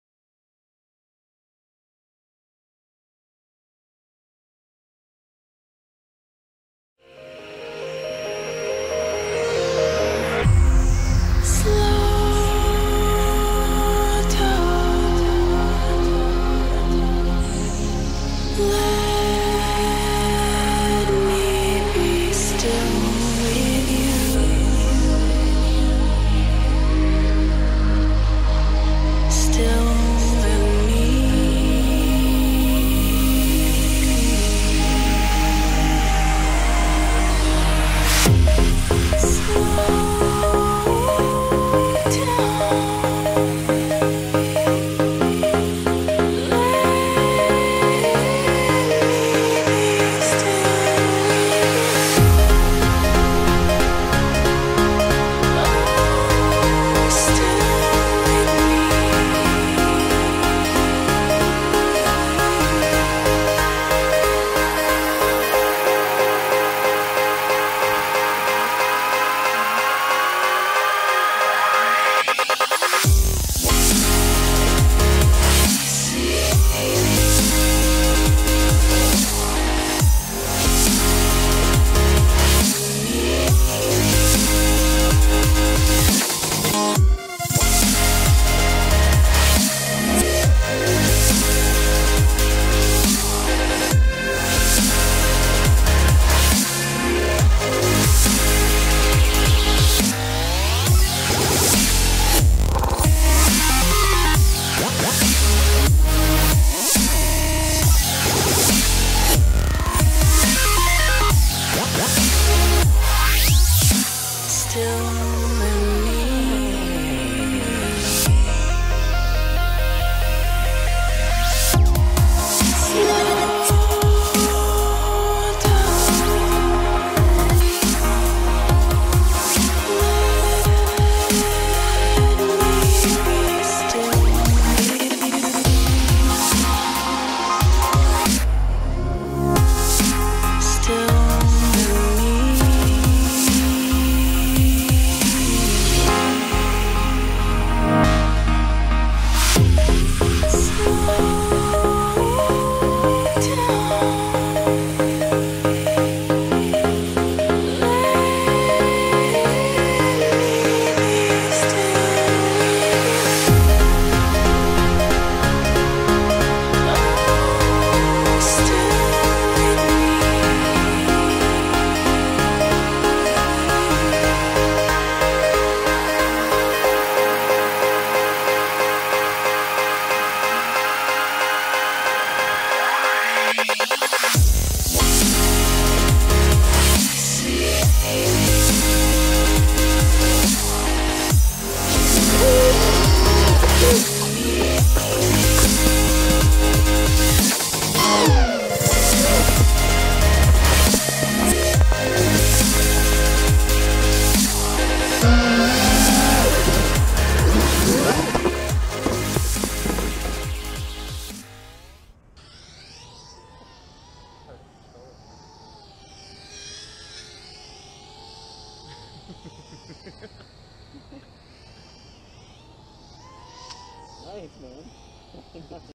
Vegas Lights with Khaos
QQ190 frame
At least we didn't get stabbed at the park.

Full Kiss setup 24A Race escs.

Killin' it after a knife fight in the ghetto.

Sometimes you gotta fly.

Catch'em all.